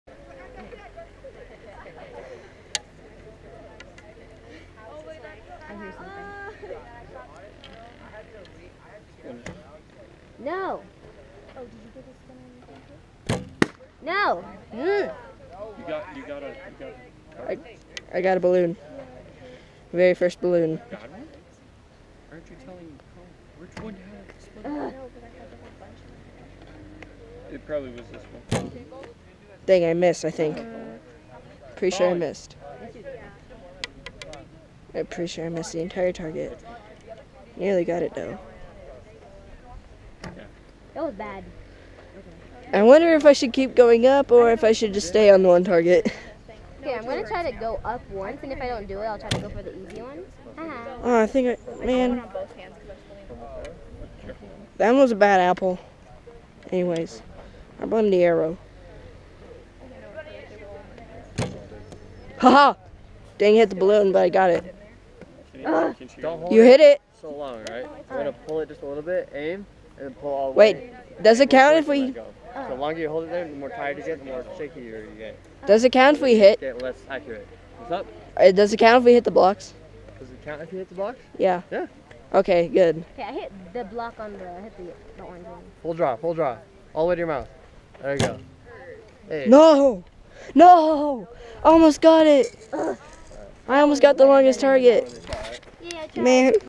challenge shooting | Archery #6 (PSR)
In this video I am shooting at up hill targets will balloons.